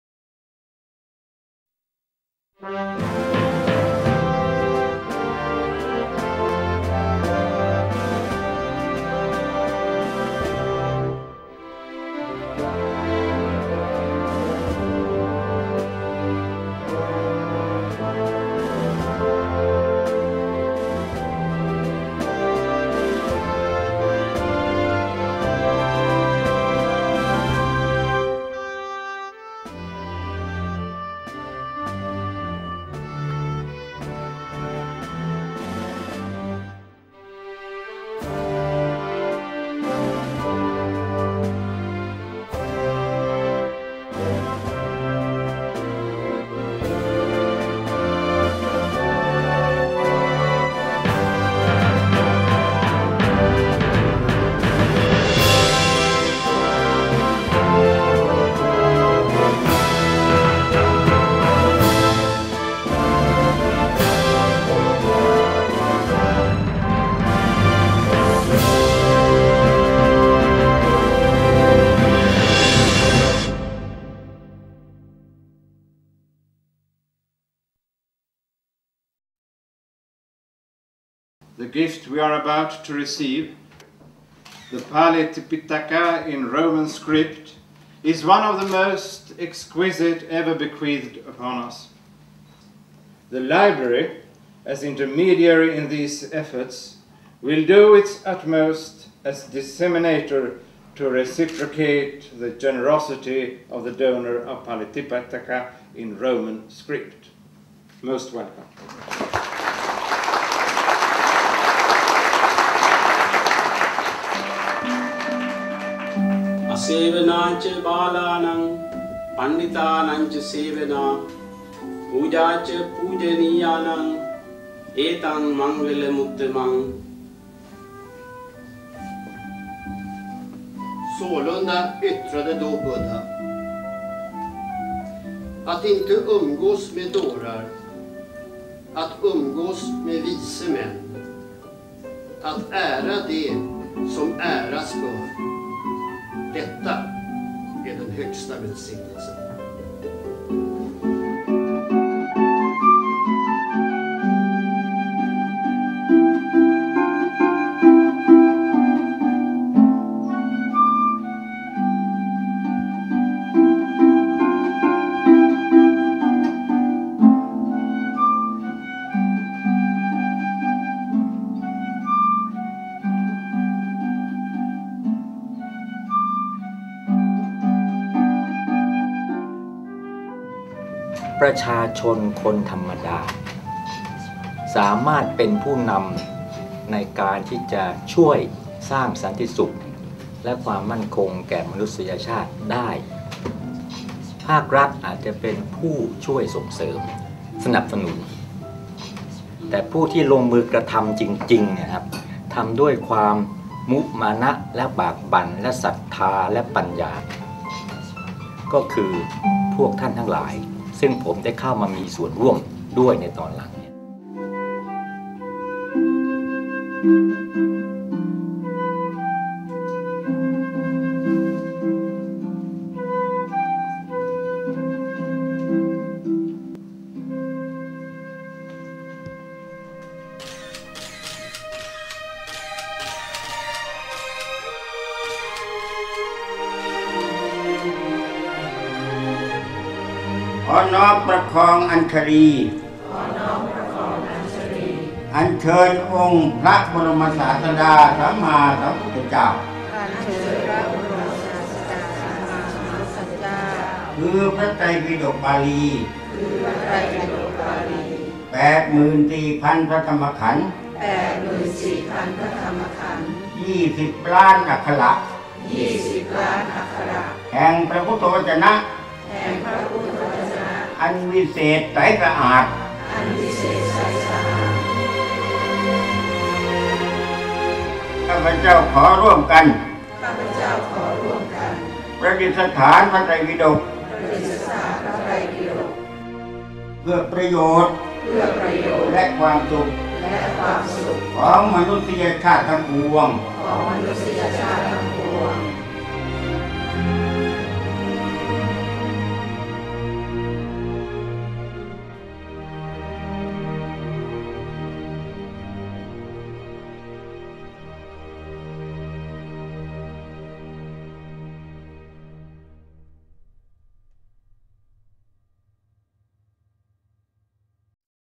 Princess Galayani's Inauguration of World Tipitaka 2005
WORLD TIPITAKA FOR SWEDEN

On March 6, 2005, Her Royal Highness Princess Galyani Vadhana, Honorary President of Tipitaka Presentation Worldwide, graciously presented an inaugural World Tipitaka Edition to the Presiden of Sri Lanka in Colombo upon a presidential invitation.  On August 15, 2005, another inaugural World Tipitaka Edition was graciously presented to the President of the Constitutional Court of Thailand in Bangkok.

In the West, the Kingdom of Sweden has proved itself, domestically and internationally, to be the promoter of peace and wisdom-based deveopment, as well as having been in the forefront of intelligent, responsible social and environmental protection. The Dhamma Society discerned that the Kingdom of Sweden would be the first and the most appropriated country in which to entrust the most Sacred Text of Theravada Buddhasasana, for preservation, and for development  of a truely wisdom-based culture.

In 2005, Her Royal Highness Princess Galyani Vadhana, the Royal Matriarch and Honorary President of the World Tipitaka Project graciously presented a special 40-volume inaugural Tipitaka edition to Uppsala University upon a formal request from the University Vice Chancellor and Chief Librarian of the Carolina Reiviva.  The Royal Presentation Ceremony took place in Bangkok on August 15, 2005 and on September 13, patron members of the Dhamma Society made a Tipitaka  Pilgrimage to Sweden to hand-over the royal gift of Tipitaka in Roman script to the Library Carolina Rediviva, Uppsala University, Uppsala, in Sweden.

With this smooth and simple handing-over process, the World Tipitaka Edition, after 50 years from its first inception of the Great International Council in Burma, including 6 years of recent proof-reading as well as database-formating and printing in Thailand, has come to a happy conclusion in the west.

Pali Tipitaka is a gift of Dhamma and Peace for the World. Dhamma Society welcomes participation and cooperation from Peace-loving international community worldwide.

Digital Archives form the M.L. Maniratana Bunnag Dhamma Society Fund's World Tipitaka Project in Roman script, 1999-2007.